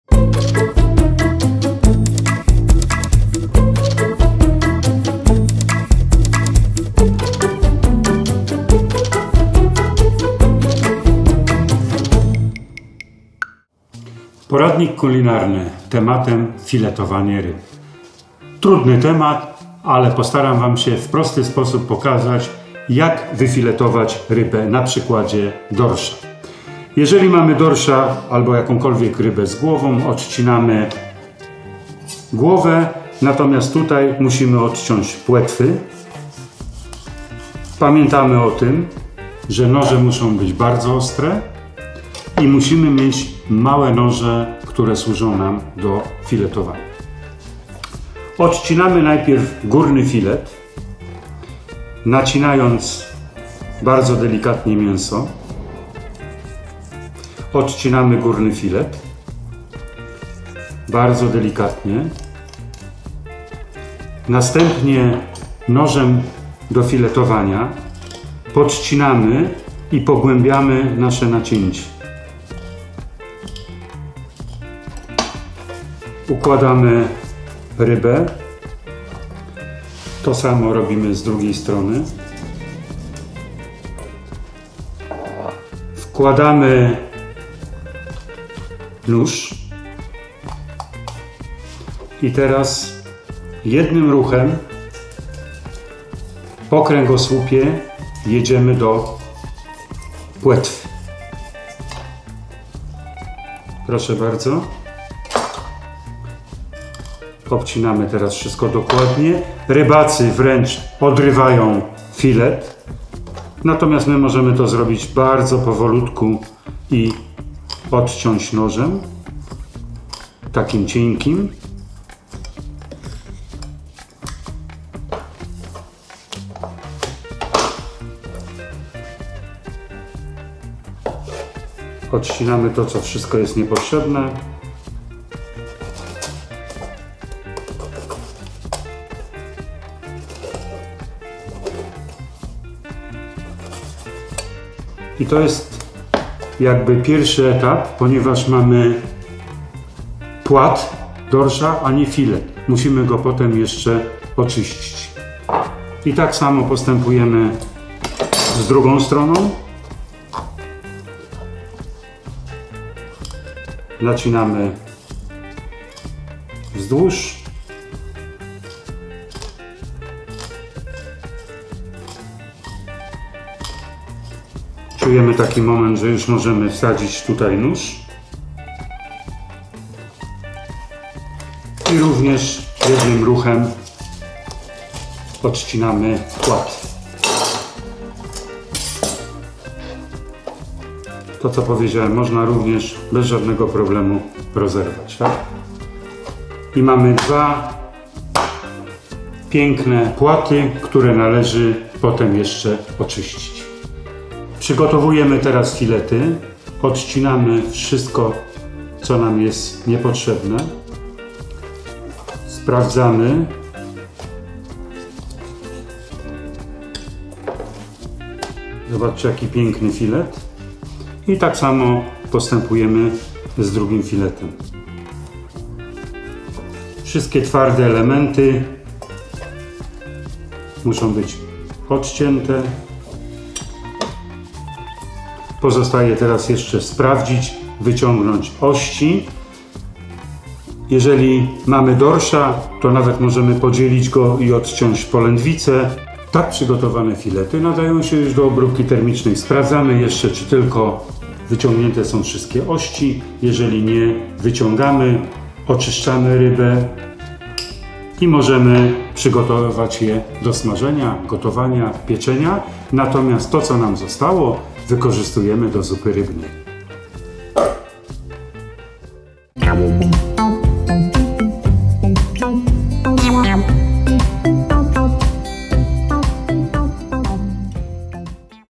Poradnik Kulinarny odc. 13 - Jak filetować rybę?
Filetowanie ryb to sztuka. Jak poprawnie to robić zobaczycie w materiale przygotowanym przez Leszka Wodnickiego.

Ten i więcej materiałów znajdziecie na naszej stronie internetowej

Projekt współfinansowany jest ze środków Europejskiego Funduszu Morskiego i Rybackiego w ramach Programu Operacyjnego „Rybactwo i Morze” na lata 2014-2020.